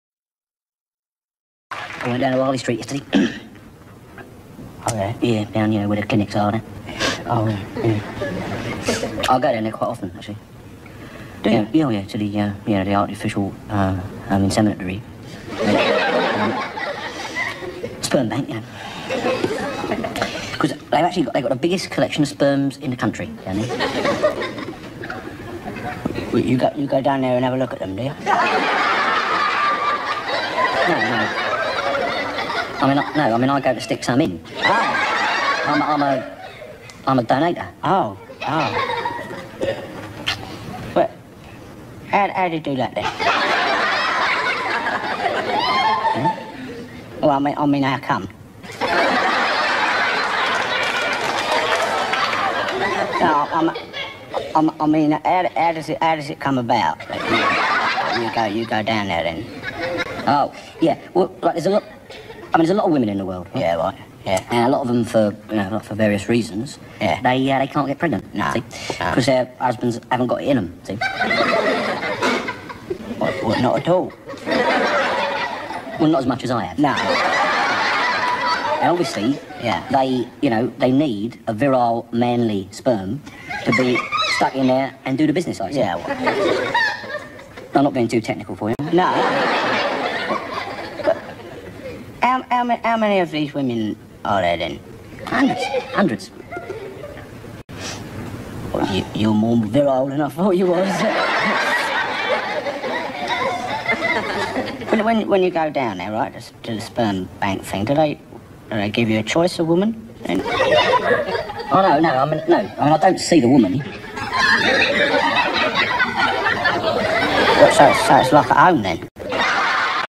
WHEELCHAIR WARRIORS Bar Talk with HILARIOUS Comedy! (SPERM BANK)
Going to the sperm bank